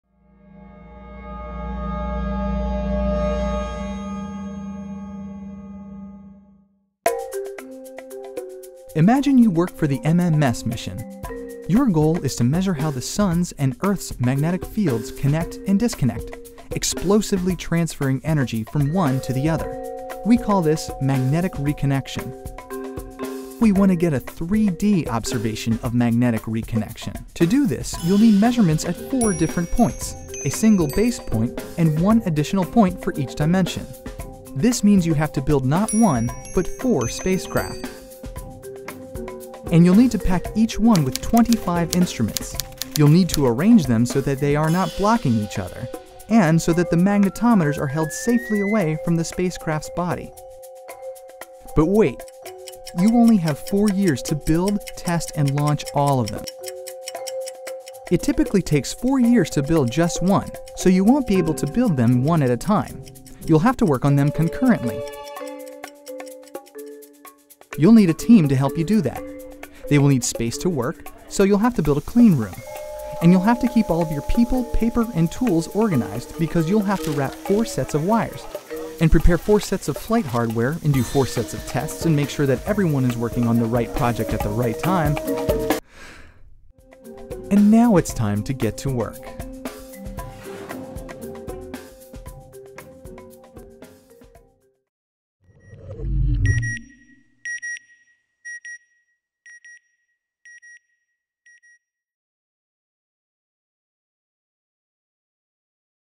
NASA | MMS Engineering Challenges
It's hard enough to build one spacecraft, but the Magnetospheric Multiscale Mission (MMS) is building four. Together, the spacecraft will unlock the mysteries of magnetic reconnection, when magnetic fields explosively connect and disconnect, transferring energy. MMS will measure reconnection between the sun's and Earth's magnetic fields. We want to learn more about magnetic reconnection because it can affect systems like GPS, radio communication, and electrical power grids.